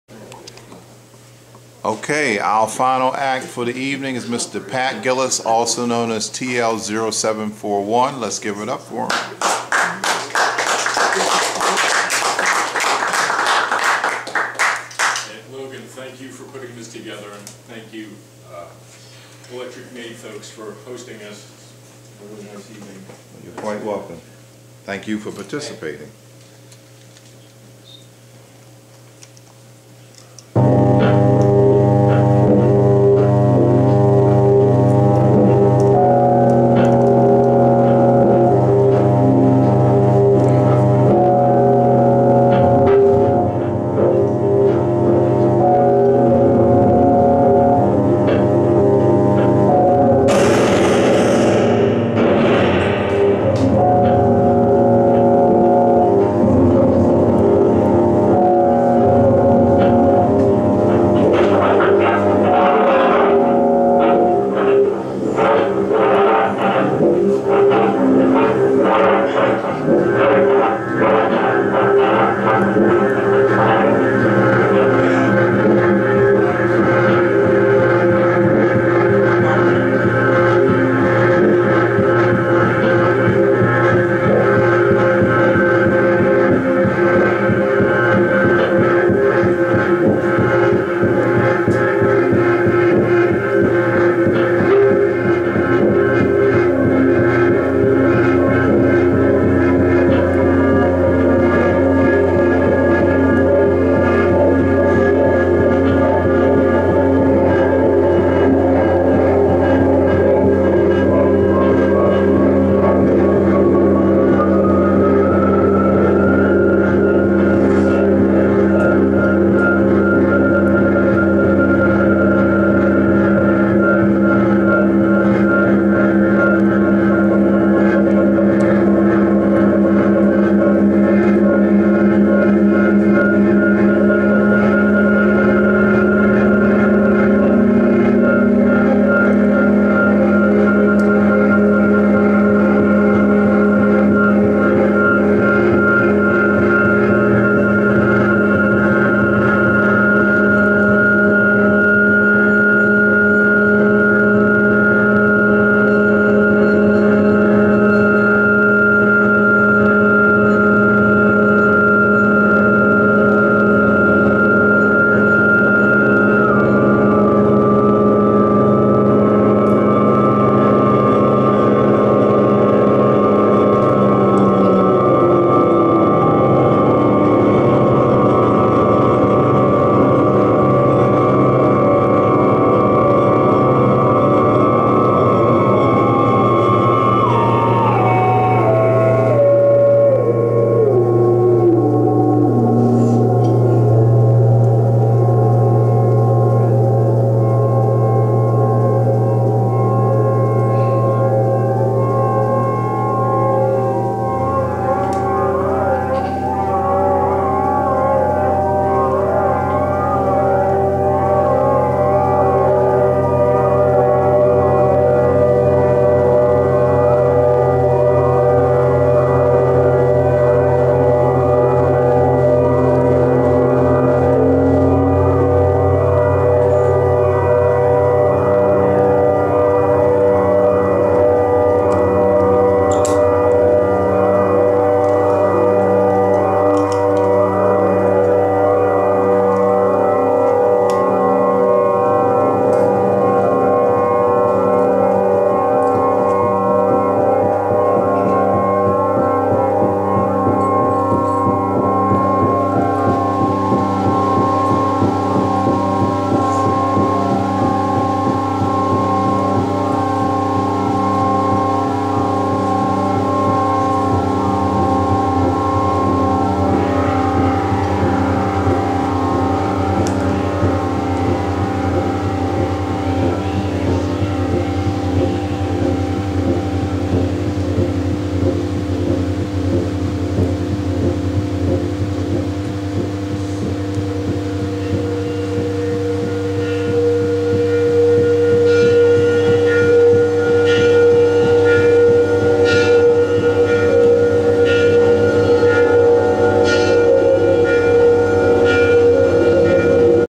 Pat Gillis's performance on 07/08/2017 (Hi-Def video 5 of 5)
Baltimore Electronic Music 2017 Summerfest Concert at the Electric Maid in Washington DC on 07/08/2017

Logan Mitchell Sr - Baltimore SDIY Group's leader & founder
Videos Produced by Synth Tech Project Productions